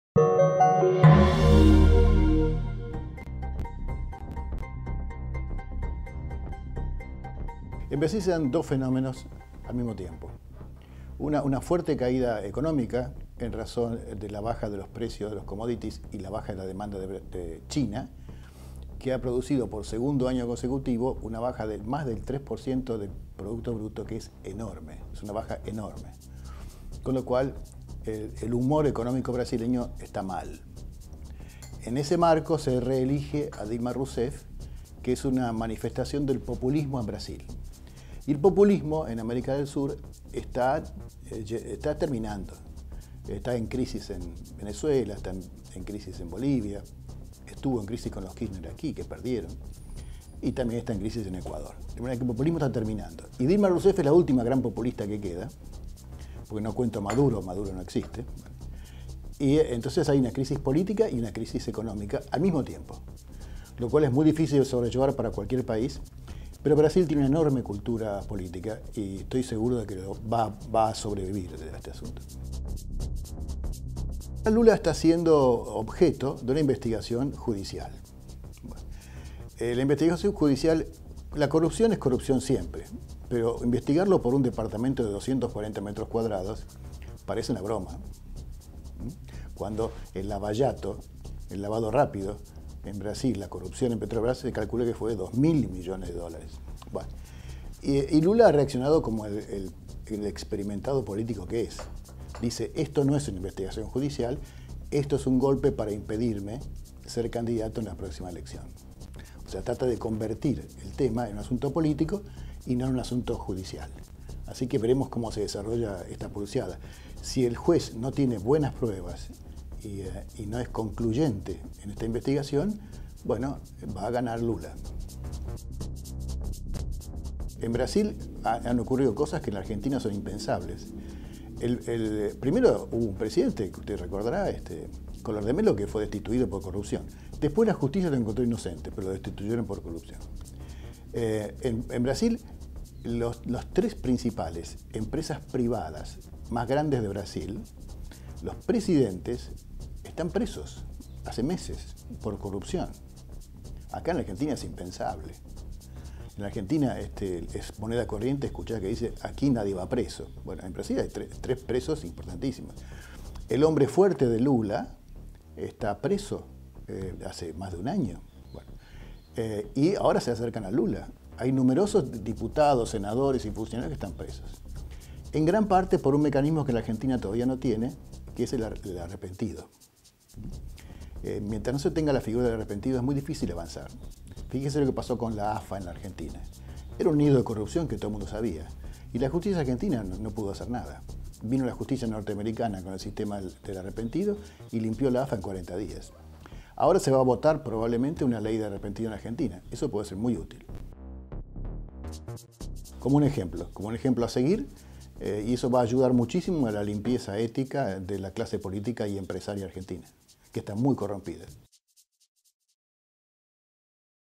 La crisis en Brasil y su impacto en la Argentina
El ex vicecanciller Andrés Cisneros evaluó las dificultades económicas y políticas que afronta el país vecino. Consideró que las causas contra políticos y empresarios por corrupción pueden ser un 'ejemplo' para la Argentina.